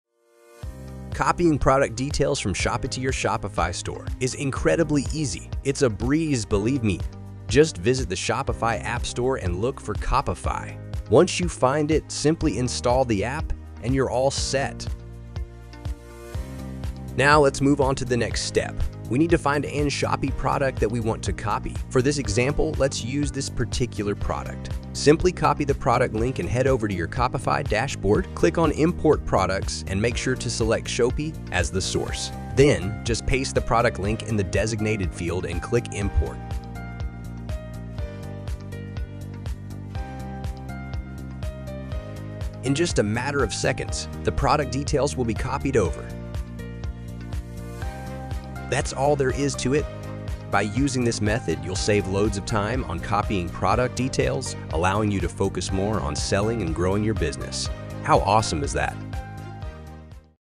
How to copy Shopee products to Shopify (Easy method)
In this step-by-step tutorial, we'll show you an easy method to copy products from Shopee to Shopify effortlessly! If you're an online seller looking to expand your reach, this is the perfect guide for you. We'll walk you through the entire process, enabling you to replicate your Shopee product listings on Shopify in no time.